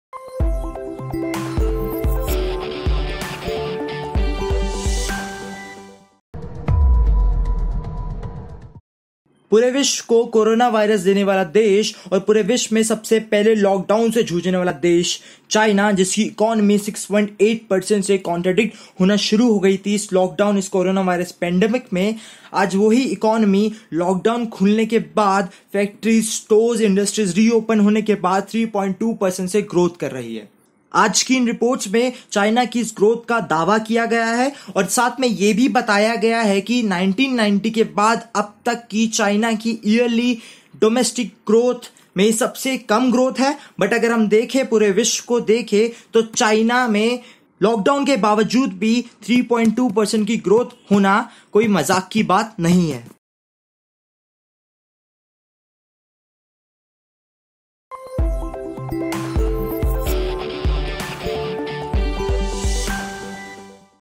INTERNATIONALNEWS China's dramatic improvement in this pandemic time
News@40 sec
China has surprised the world by reporting significant positive growth despite being hit hard by the coronavirus pandemic.
The expansion reported Thursday was a dramatic improvement over the previous quarter’s 6.8%contraction — China’s worst performance since at least the mid-1960s. But it still was the weakest positive figure since China started reporting quarterly growth in the early 1990s.